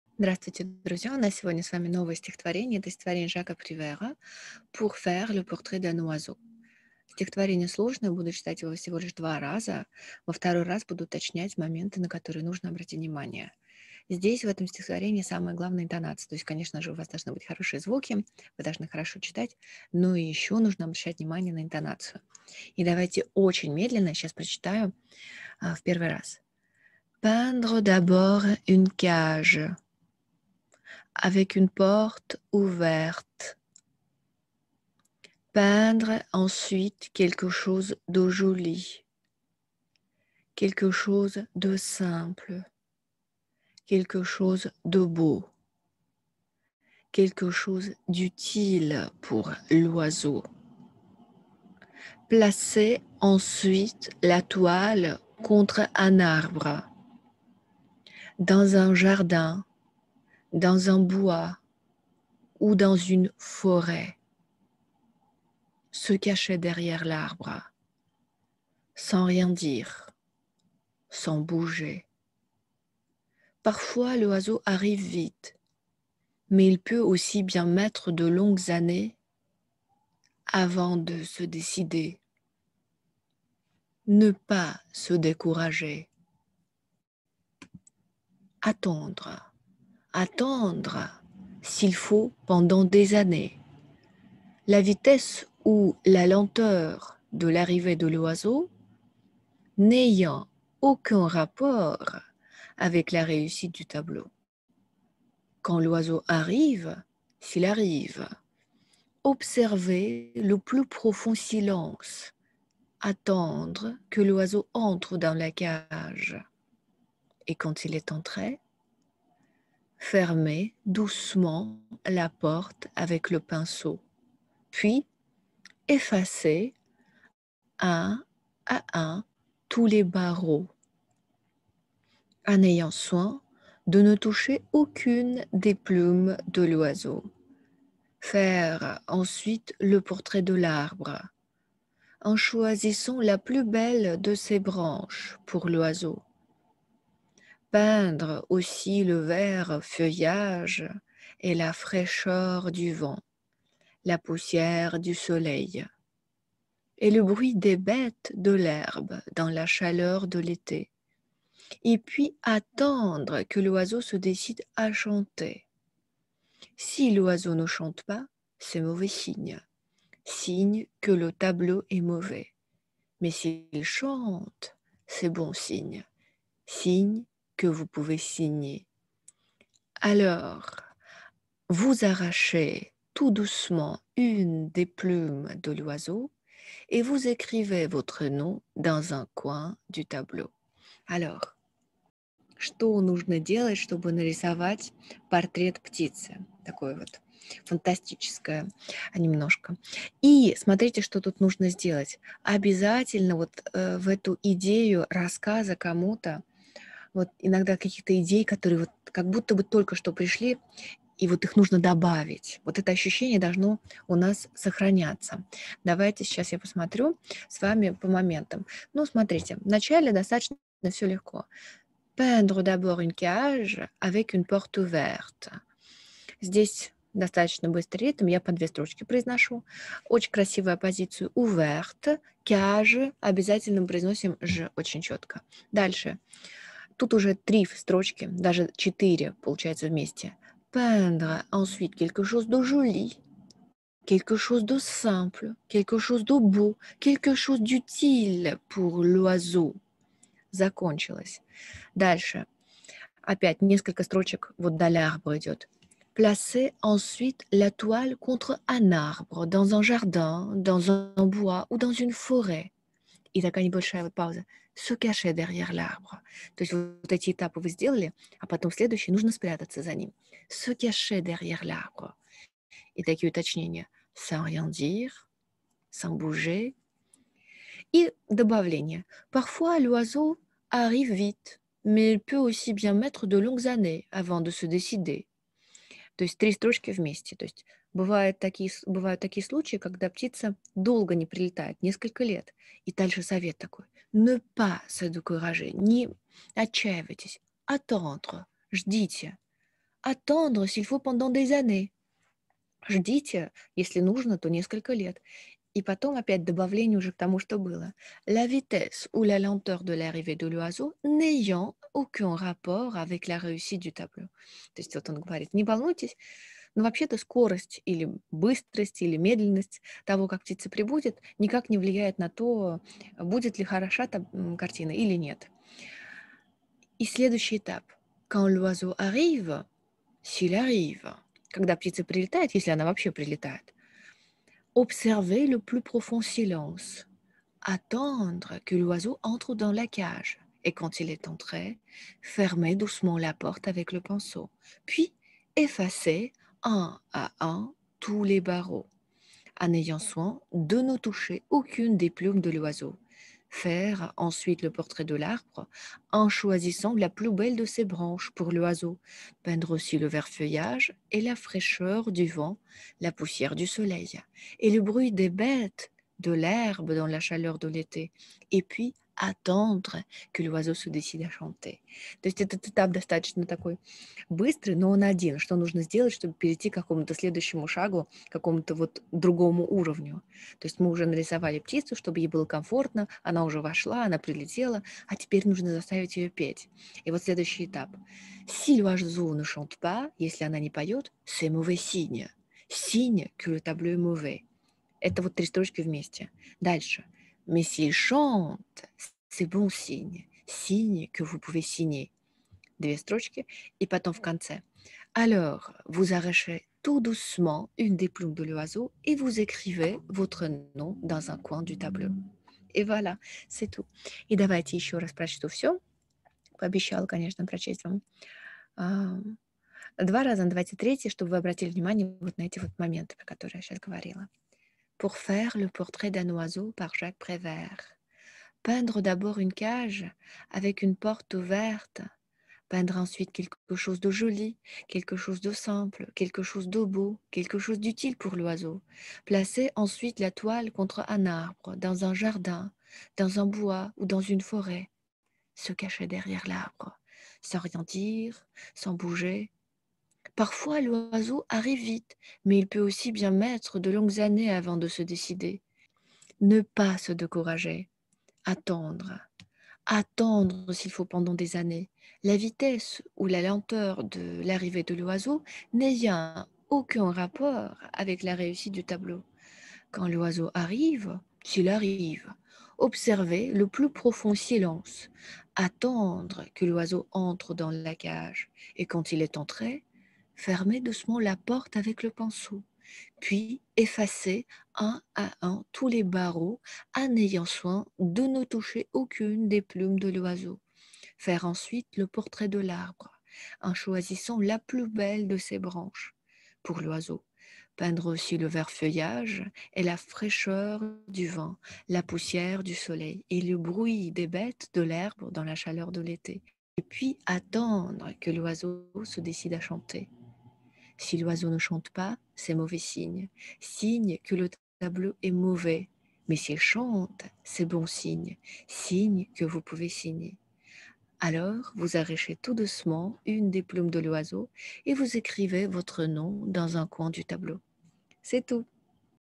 Стихотворение на французском c разбором №45 "Pour faire le portrait d'un oiseau"
Pour faire le portrait d'un oiseau par Jacques Prévert

1) Медленное чтение.
2) Чтение по строчке.
3) Быстрое чтение.